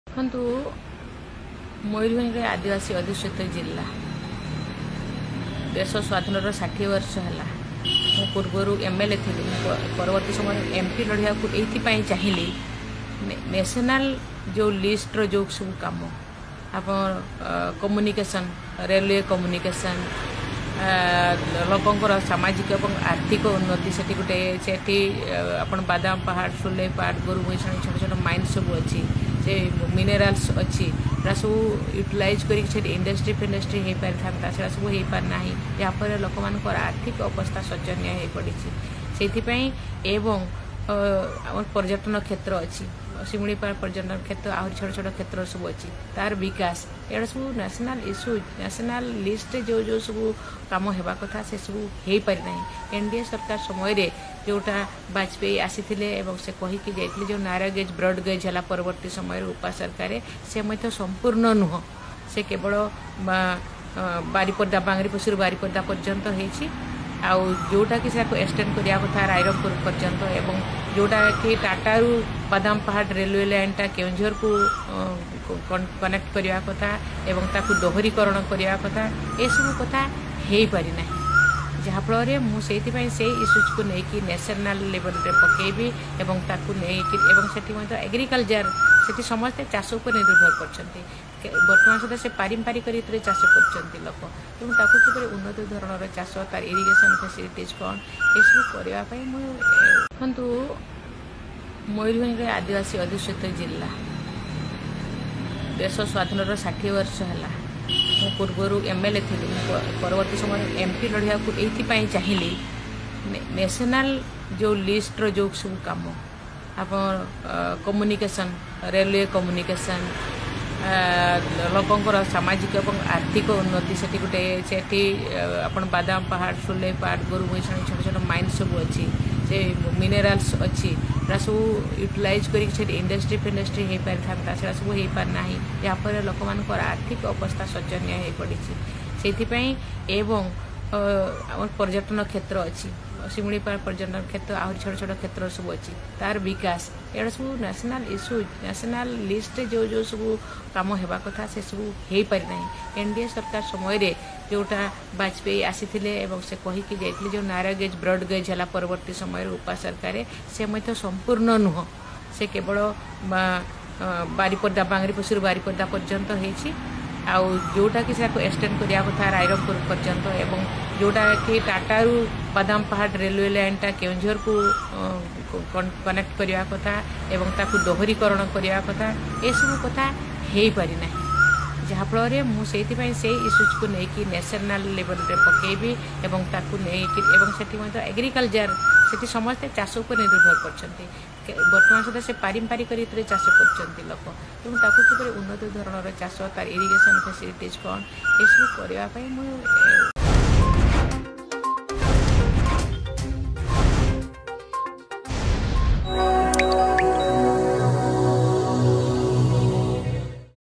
What Draupadi Murmu spoke 13 years back, watch her rare interview
13 years back Draupadi Murmu contested for the Mayurbhanj Lok Sabha seat. This is her interview taken at that time. Then also she was the symbol of courage and determination.